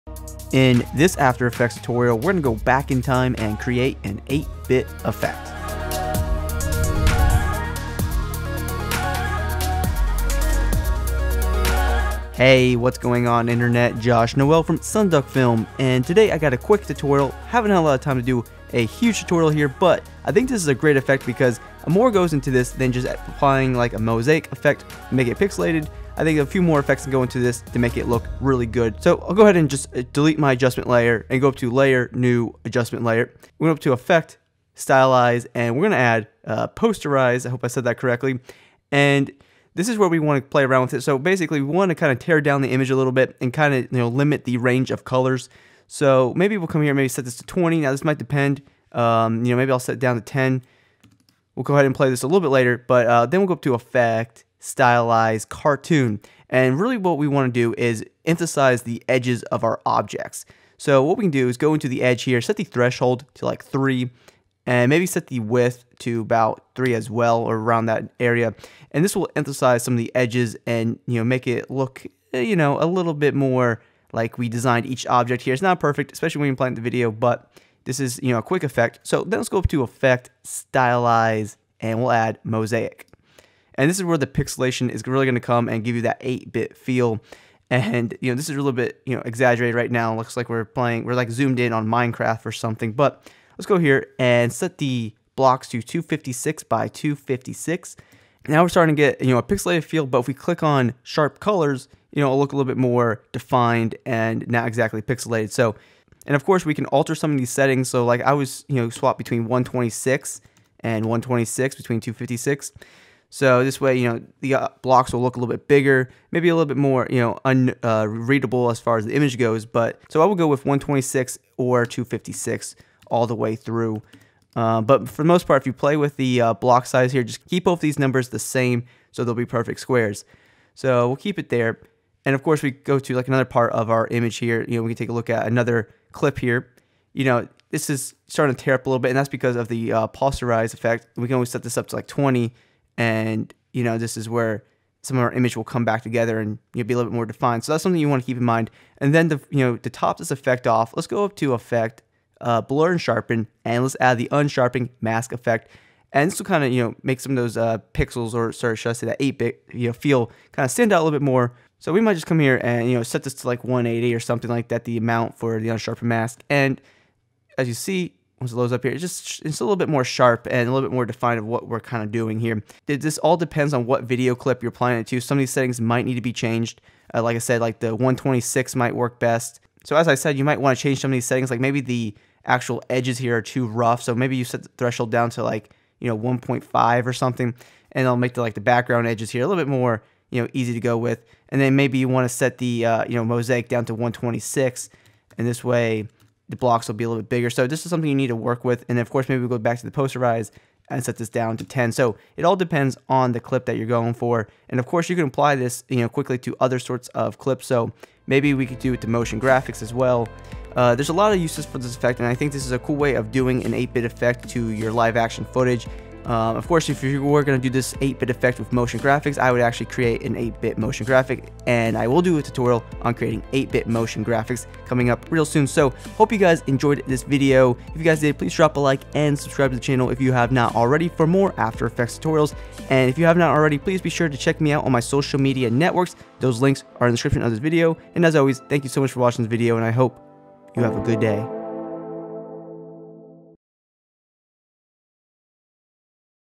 Create the 8BIT EFFECT for Video Footage | After Effects Tutorial
In this Adobe After Effects Tutorial, create an 8bit effect for your video footage. Creating the 8bit effect is extremely easy. We use the effects posterize, cartoon, mosaic, and unsharpen mask. Posterize limits the range of colors. Cartoon is used to bring out the edges around your objects. Mosaic is used for the creation of blocks. Then finally, unsharpen mask is used to help bring out the blocks. After watching this After Effects Tutorial, you will be able to quickly recreate the 8bit effect for your video footage!

Download Project Files Here:
Uploading Today!